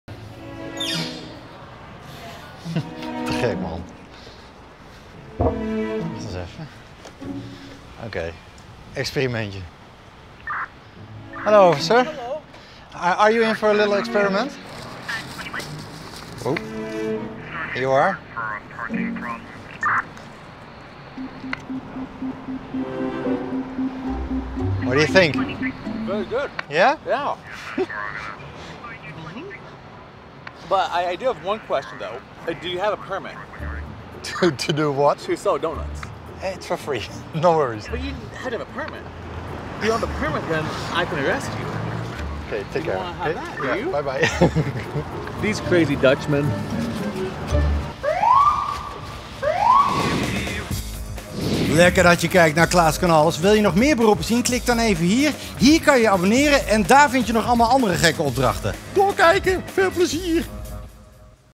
'Agent, wilt u meedoen aan experiment?' - Kan Klaas voor één dag donutbakker zijn?
Klaas heeft donuts gemaakt en tja de nummer 1 fan van donuts zijn natuurlijk Amerikaanse politieagenten, dus Klaas laat ze testen!